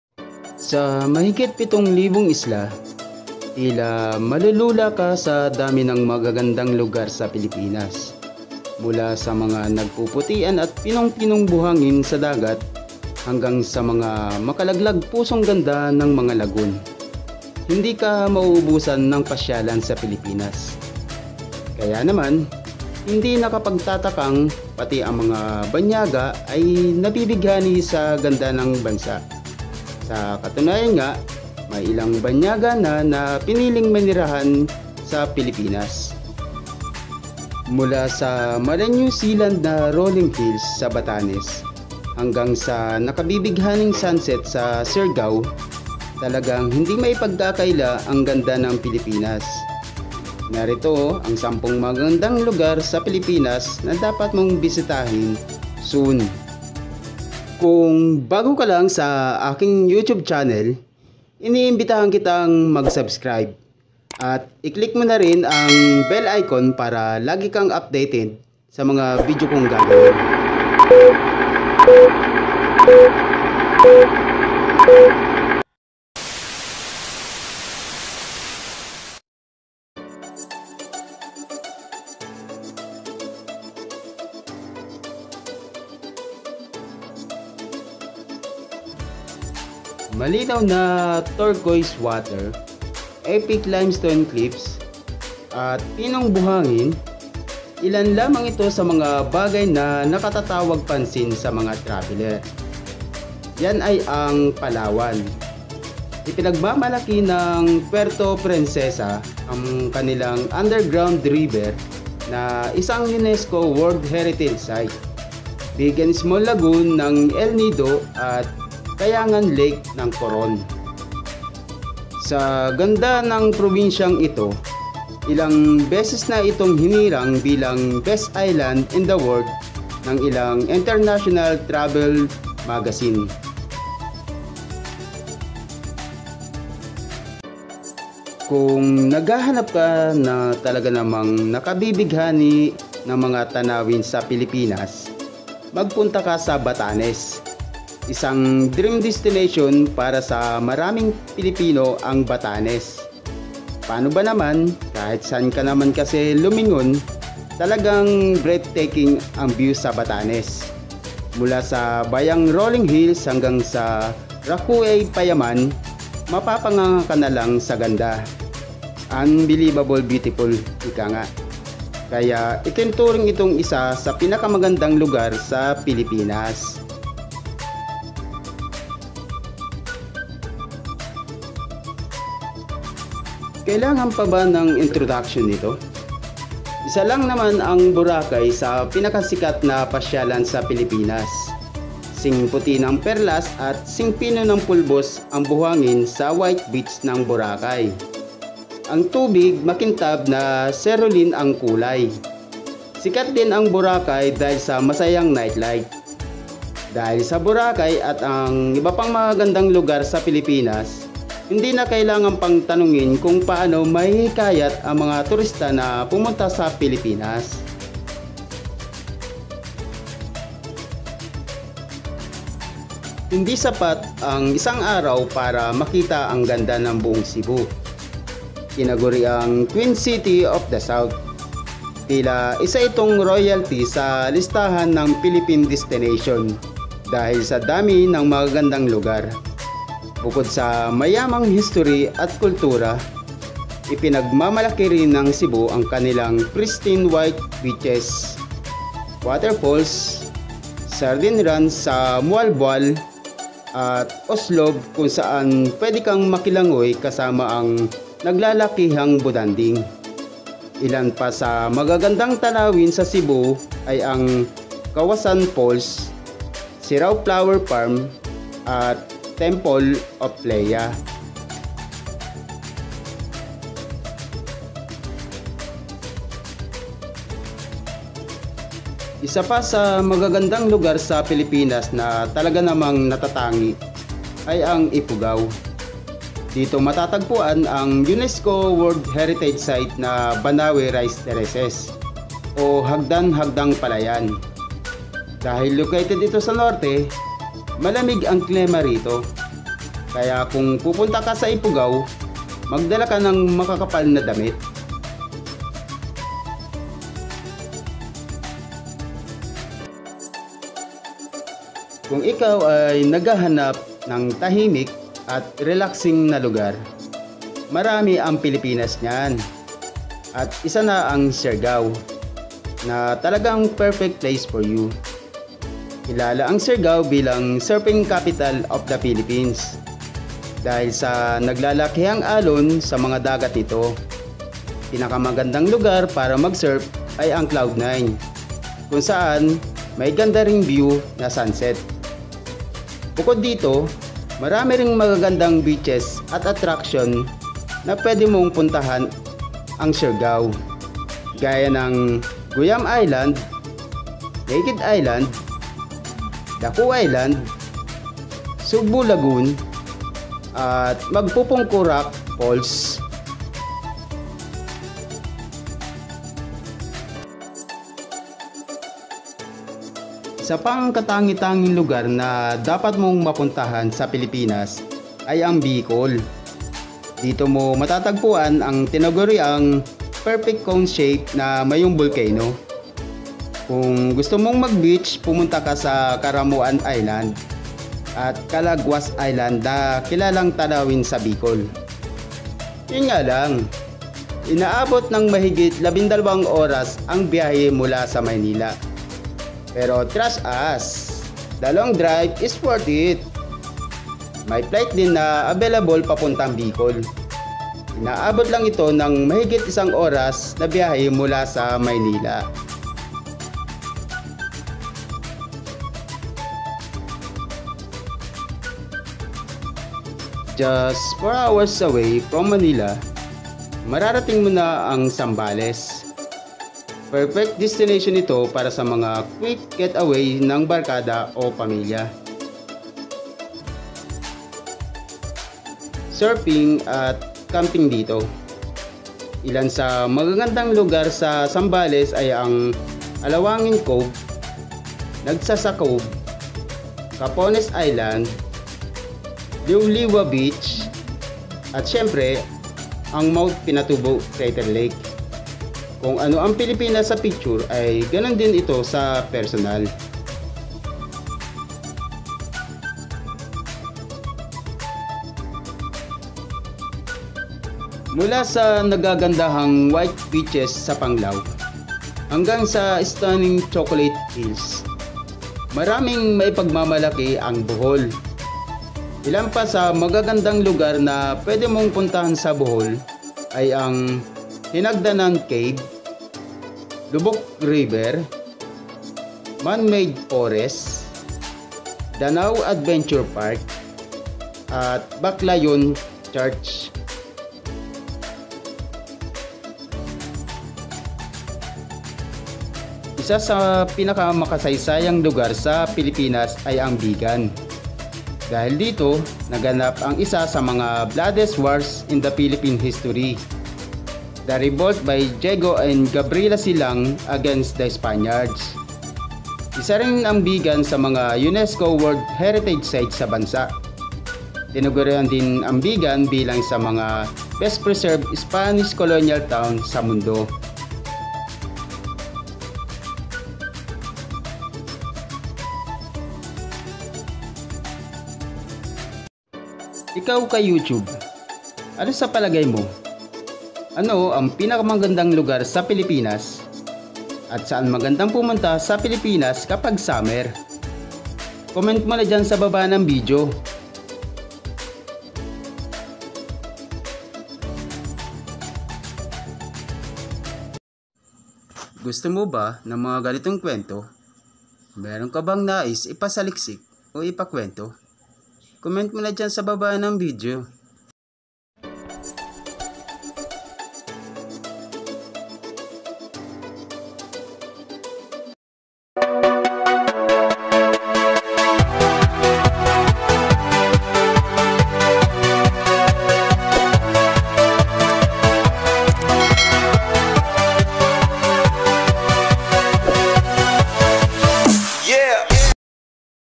10 Pinakamagandang Lugar sa Pilipinas.
10 Pinakamagandang Lugar sa Pilipinas
       Mula sa mala New Zealand na Rolling Hills sa Batanes hanggang sa nakabibighaning sunset sa Siargao, talagang hindi mapagkakaila ang ganda ng Pilipnas. Narito ang sampung magagandang lugar sa Pilipinas na dapat mong bisitahin soon!

1.PALAWAN
*Undergroud River - UNESCO World Heritage Site
*El Nido - Big and Small Lagoon
*Coron - Kayangan Lake

2.BATANES
*Vayang Rolling Hills
*Racuh A Payaman
*Batanes Light Tower

3.BORACAY
*White Beach
*Boracay Nightlife

4.CEBU
*Pristine White Beaches
*Waterfalls
   -Kawasan Falls
*Sardine Run - Moalboal
*Oslob
*Sirao Flower Farm
*Temple of Leah

5.IFUGAO
*Banaue Rice Terraces - UNESCO World Heritage Site

6.SIARGAO
*Surfing Capital of the Philippines
  -Cloud 9
*Sunset View
*Guyam Island
*Naked Island
*Daku Island
*Sugba Lagoon
*Magpupungko Rock Pools

7.BICOL
*Mayon Volcano - perfect cone shaped
*Caramoan Island
*Calaguas Island

8.ZAMBALES
*Anawangin Cove
*Nagsasa Cove
*Capones Island
*Liwliwa Beach
*Mt.Pinatubo Crater Lake

9.BOHOL
*Panglao Beach
*Chocolate Hills
*Hinagdanan Cave
*Loboc River
*Man-made Forest
*Danao Adventure Park
*Baclayon Church

10.VIGAN
*Vigan Old Houses -UNESCO World Heritage Site
- Best Preserved Spanish Colonial Town

Ngayon nakita mo na ang 10 Pinakamagandang Lugar sa Pilipinas?
Ano at Saan ka magandang pumunta ngayung Summer?
Comment mo na dyan sa baba ng Video.

©To the Original owner of the Music.

-ARCH tv